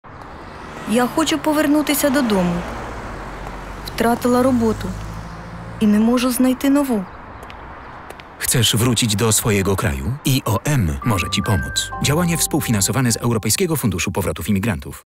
IOM - Program Pomocy w Dobrowolnym Powrocie i Reintegracji
Informacja na temat Programu Pomocy w Dobrowolnym Powrocie i Reintegracji trafiać ma także do sąsiadów i przyjaciół oraz osób, które mają kontakt z migrantami.